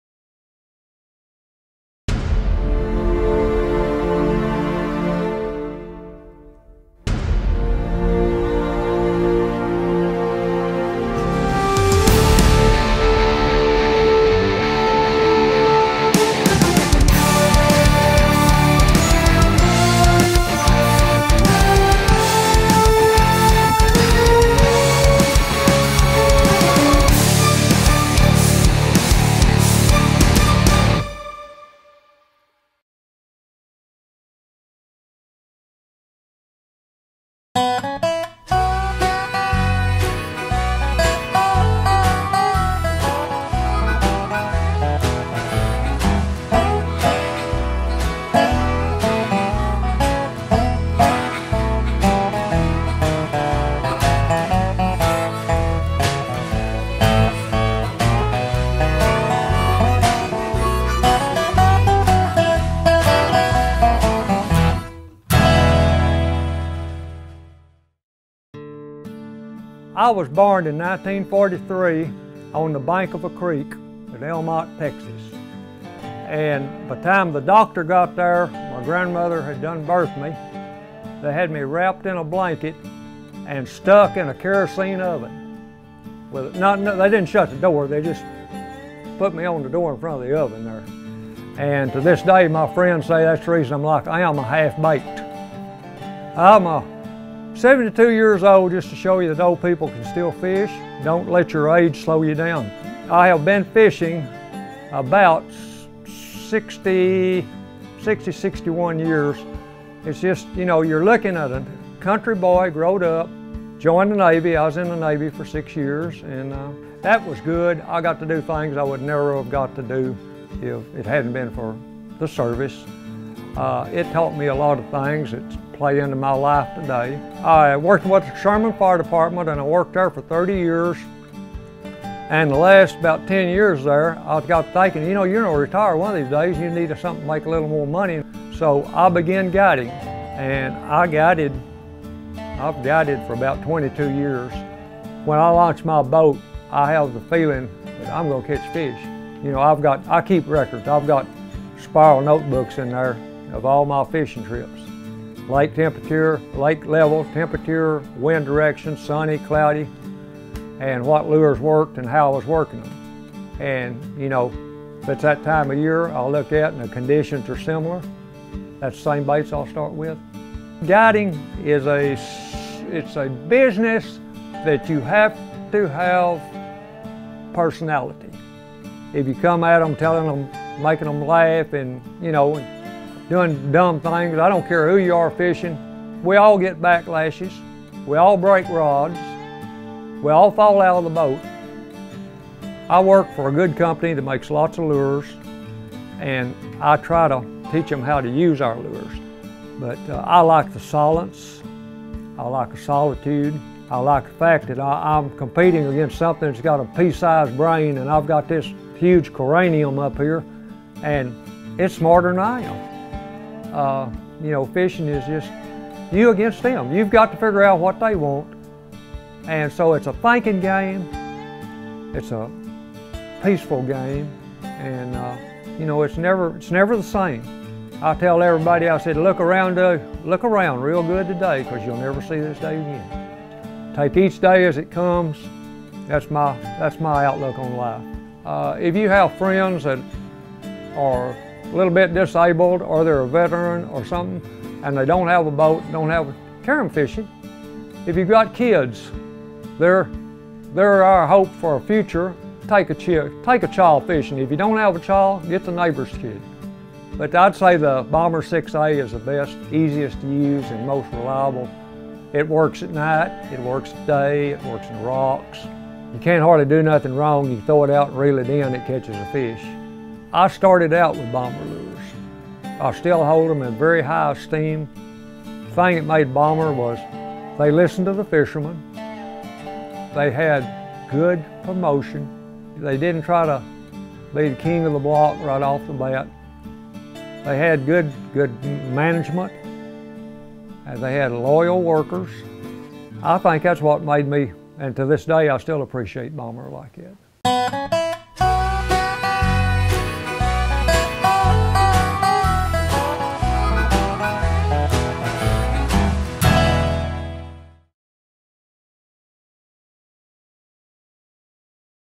Bomber All Water | Half Baked
Join us as we highlight another one of the characters from Bomber’s History. Texas fishing guide JB Webb doesn’t let age slow him down. A retired firefighter with over 61 years of fishing experience, the silence and solitude of fishing have captivated his attention nearly his entire life. “Fishing is a thinking game, it’s a peaceful game, and it’s never the same.”